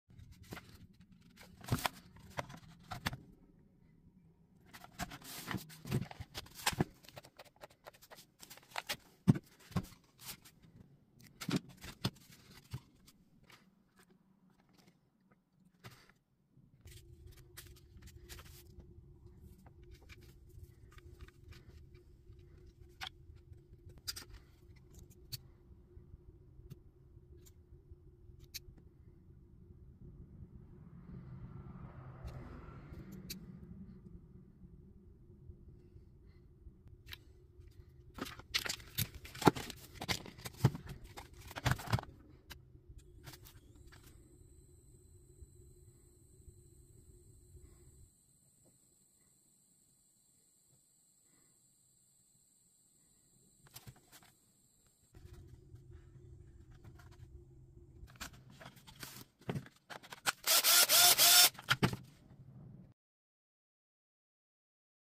DEAD Milwaukee Battery Quick Fix
In this video, I tackled a challenging repair on a Milwaukee 4Ah battery that wouldn't charge or power any tool, only reading 5.4V on the multimeter. Instead of discarding it, I used an innovative technique to bring it back to life. By connecting the terminals of a good Dewalt 5Ah battery to the Milwaukee battery, I boosted it to a chargeable range, restoring its functionality.

Join me as I walk you through the step-by-step process of this repair, explaining the methods and safety precautions to ensure a successful and safe battery revival. This video is perfect for those looking to extend the life of their battery packs and save money on replacements.

Key Points Covered:

Initial Diagnosis - Testing the Milwaukee battery and confirming the low voltage issue.
Setting Up the Boost - Preparing the Dewalt battery and connecting it to the Milwaukee battery terminals.
Boosting the Voltage - Step-by-step process of safely boosting the Milwaukee battery to a chargeable range.
Final Testing - Verifying the restored functionality of the Milwaukee battery by charging and testing it in a tool.

This procedure involves working with batteries and electrical connections. Please use caution and proper safety gear. Doctor Lefthandthread Media is not responsible for any injury or damage resulting from the replication of techniques shown in this video.